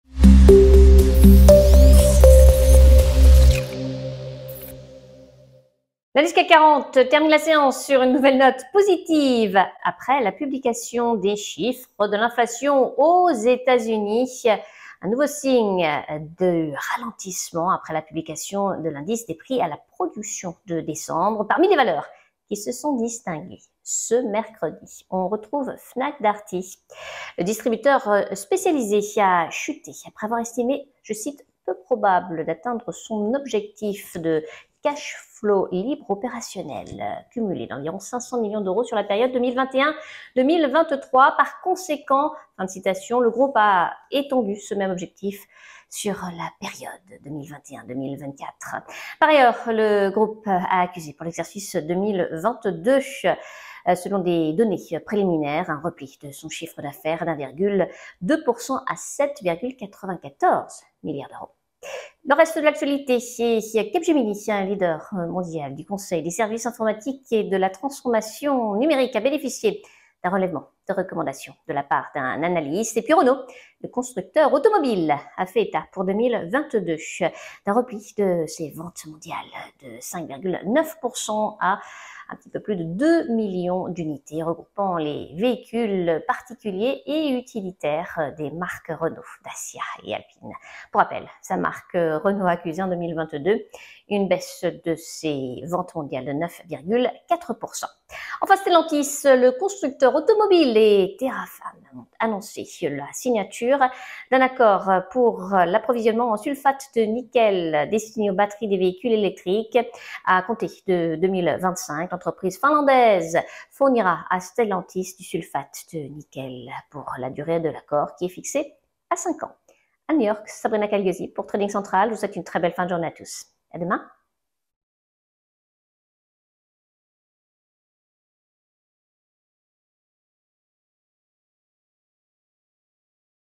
18/01 Actualité du CAC 40 en clôture: L'indice Cac40 + 0,09% FNAC Capgemini Renault Engie Stellantis
Vidéo quotidienne sur le CAC40 et sur la clôture de la Bourse de Paris de Trading Central sur Youtube: L'indice Cac 40 a gagné 0,09% à 7083.39 points mercredi après la publication des chiffres de l'inflation aux US et de l'indice des prix à la production. News, Actualités financières & boursières sur FNAC DARTY, Capgemini, Renault, Engie et Stellantis

Sommaire :
00:00 L'indice CAC 40 a gagné 0,09% à 7083.39 points mercredi et termine la séance sur une nouvelle note positive après la publication des chiffres de l'inflation aux Etats-Unis, un nouveau de signe de ralentissement après la publication de l'indice des prix à la production de décembre.
00:21 Les News & Actualités sur les valeurs qui se sont distinguées à la Bourse de Paris :
           00:25 FNAC DARTY, le distributeur spécialisé, a chuté après avoir estimé «peu probable d’atteindre son objectif de cash-flow ...
           01:03 Capgemini, un leader mondial du conseil, des services informatiques et de la transformation numérique, a bénéficié
           01:13 Renault, le constructeur automobile, a fait état pour 2022 d’un repli de ses ventes mondiales de 5.9% à 2,051,174 unités, regroupant les véhicules particuliers et utilitaires des marques Renault, Dacia et Alpine  ...
           01:39 Stellantis, le constructeur automobile, et Terrafame ont annoncé «la signature d’un accord ...

Merci, à très vite sur nos prochaines vidéos !

► Retrouvez l'ensemble de nos vidéos boursières CAC40 et Analyses Techniques : rendez-vous sur notre playlist "WEB TC - Français"
► Suivez-nous !